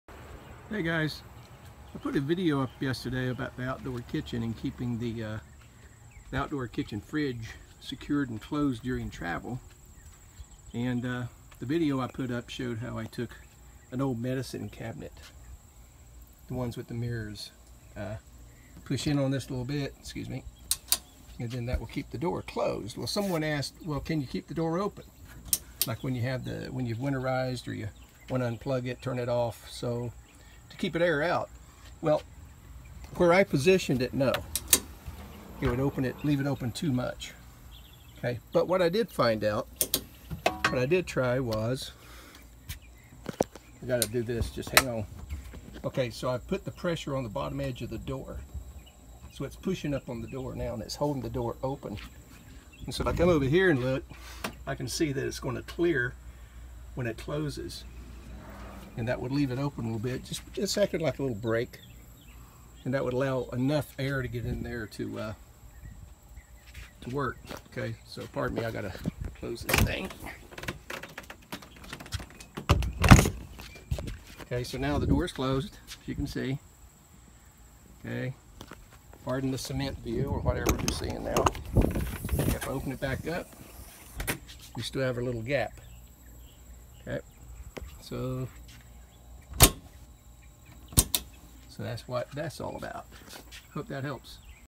RV Outside Kitchen Fridge Door-stop Alternate Use
The video I uploaded yesterday shows how my modification holds the outside kitchen fridge door closed during travel; however, the question was asked if it could be used to hold the fridge door open for airing out while the RV isn't in use with the outside kitchen hatch closed. This video shows how the modification can provide this function as well.